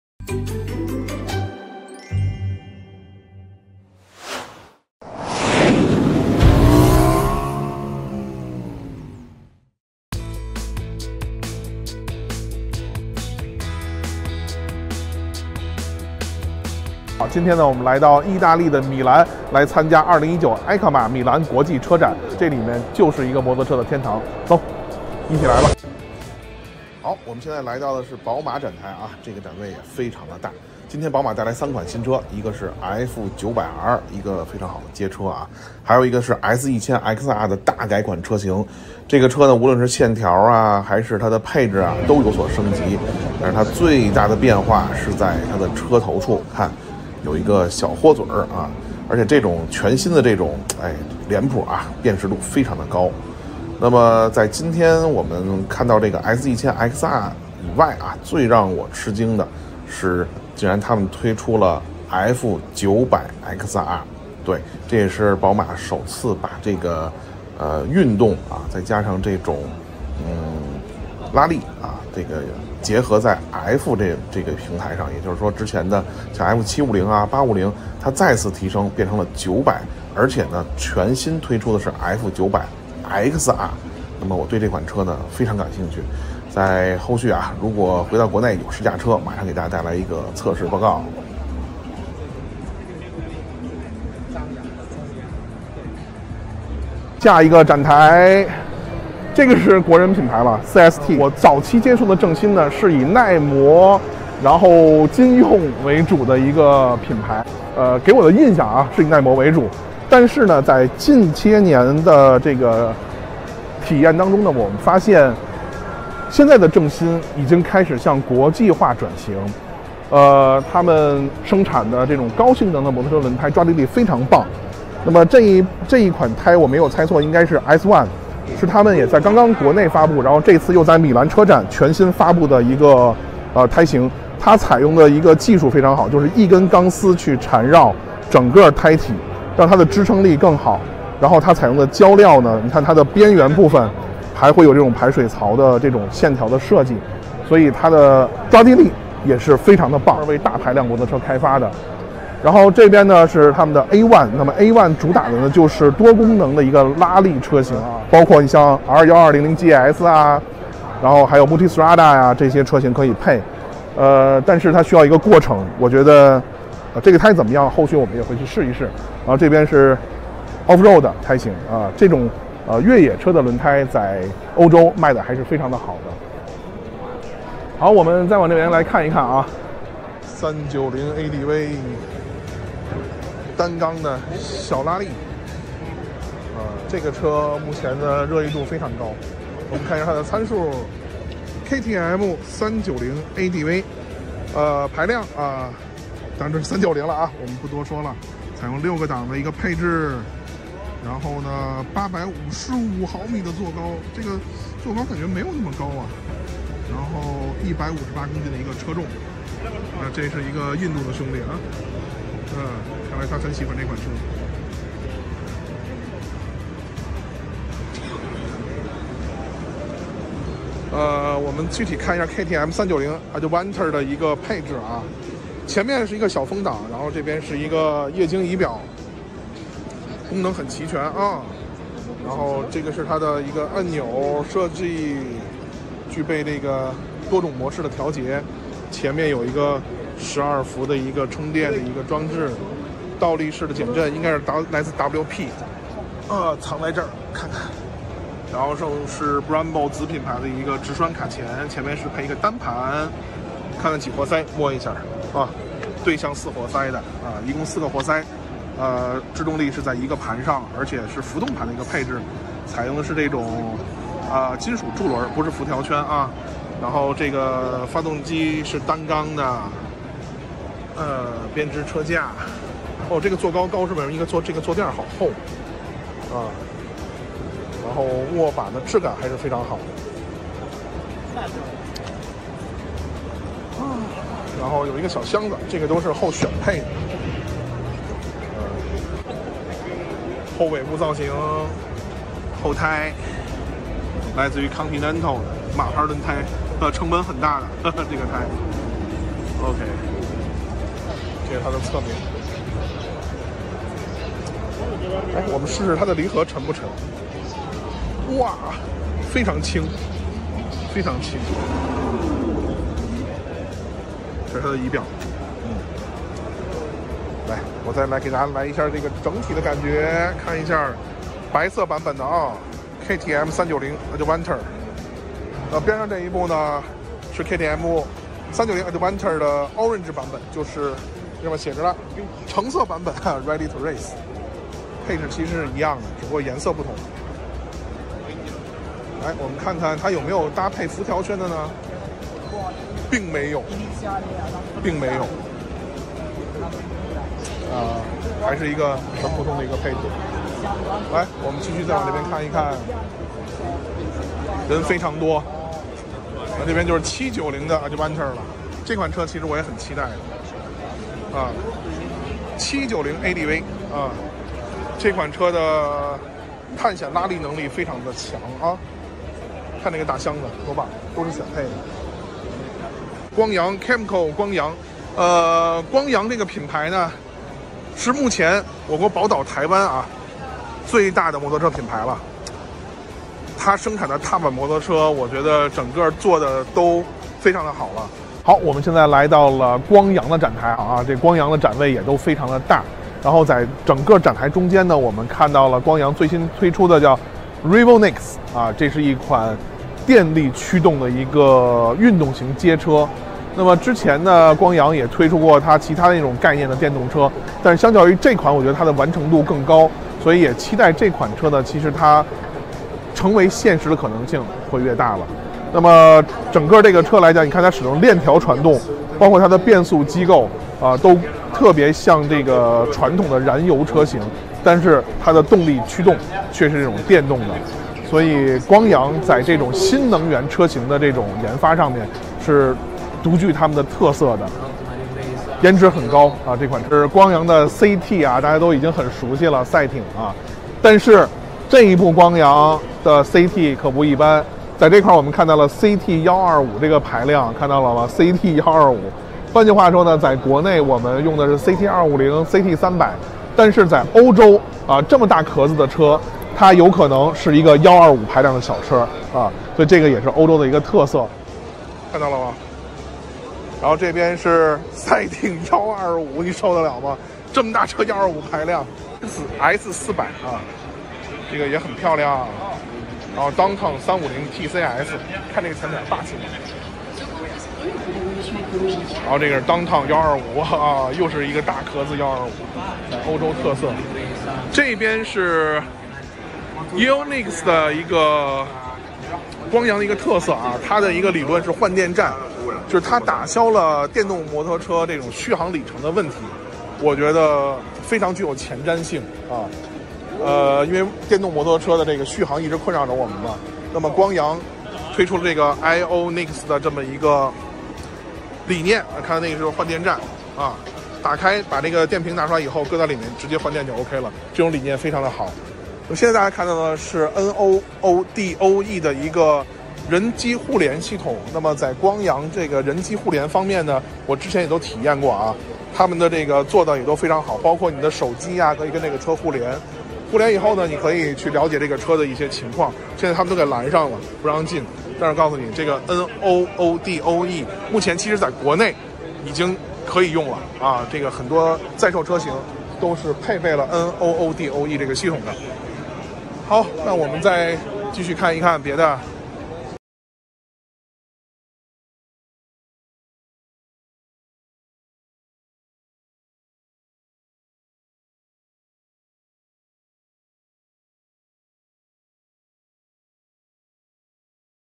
KTM 390 ADV 宝马F 900 XR等新车实拍 | 2019米兰车展
简介：在2019米兰国际车展上，BMW 推出了全新车型F 900 XR、F 900R，及大改款S 1000 XR。KTM亮相了全新390 ADV等新车。而让我感到自豪的是，华人品牌CST正新携旗下科技结晶S1以及多款高性能摩托车轮胎亮相，与国外高端品牌一争高下。来自宝岛台湾的光阳带来惊艳概念车RevoNEX以及CT125等新车，下面咱们就一起到现场看看吧！